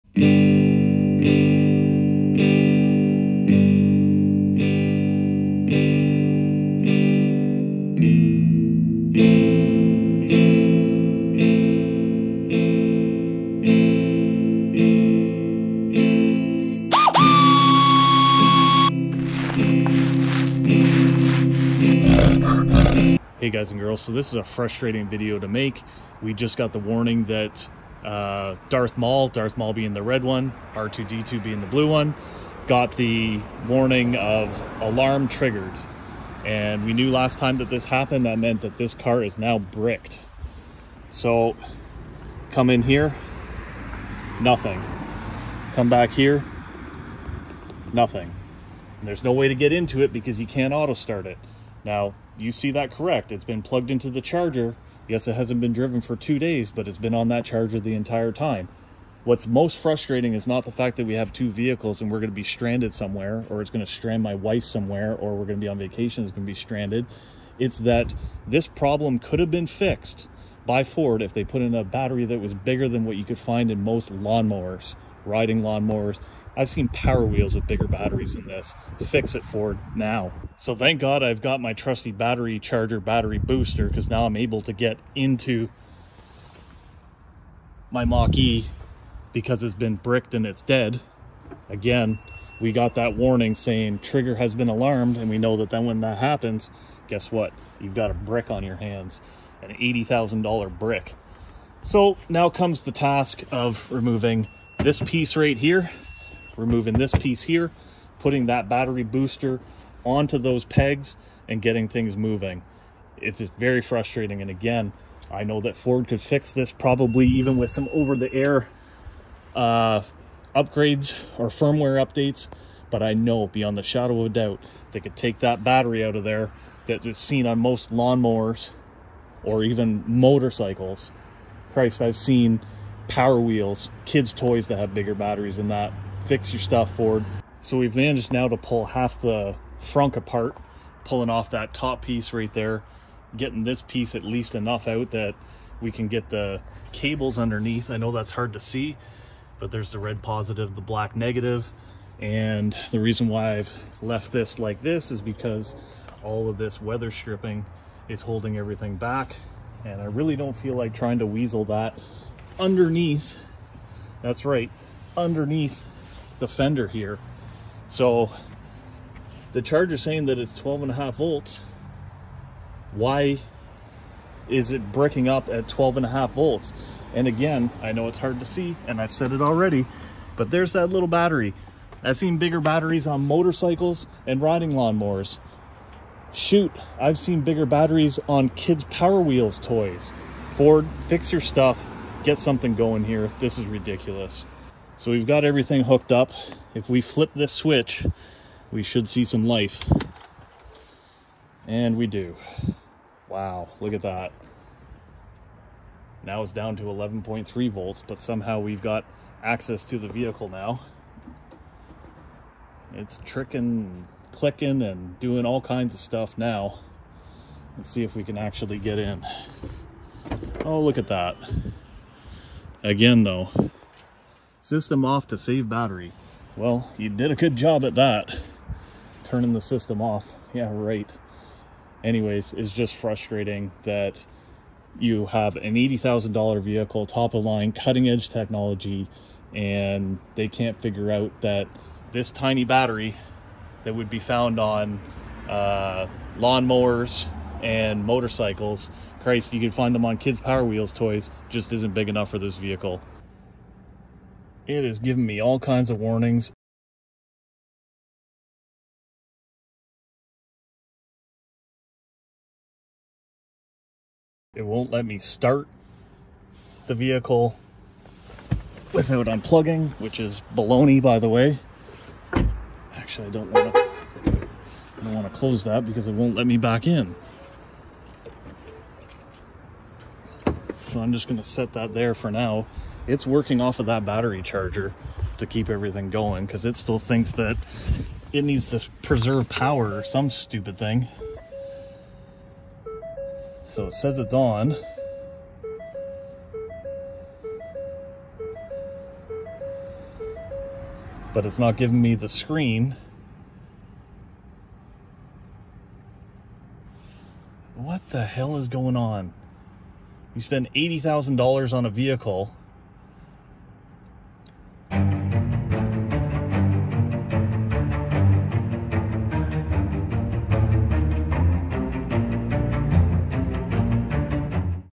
Mustang Mach E 12v battery is dead AGAIN & vehicle BRICKED || Frustrating || Ford needs fix this NOW
In this video I show you the steps involved to diagnose and fix a Dead or Bad 12v accessory battery on a Ford Mach E and F150 Lightning.  In my case it was a bad cell withing the 12v battery that was not allowing the battery to charge.

Without you, this video would not be possible.  Keep on Bearing!

Drive safe EVeryone!